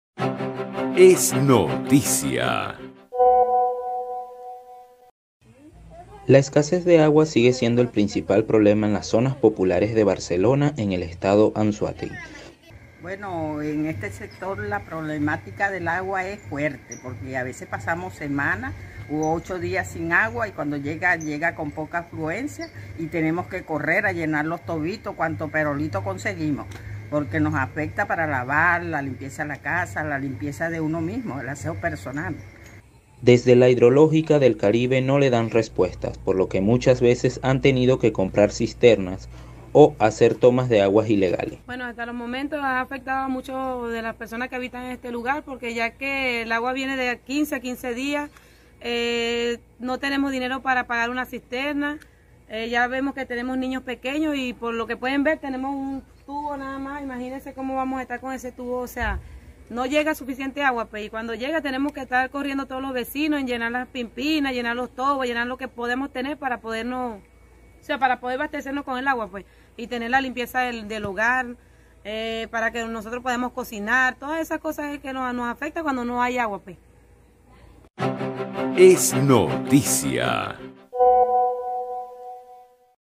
Ciudadanos en Barcelona, Anzoátegui, denuncian que no reciben agua (2.3.2021)
Ciudadanos en Barcelona, Anzoátegui, denuncian que no reciben agua.

Los vecinos reclaman que los ciclos de suministro son de casi dos semanas; afectando sus actividades diarias.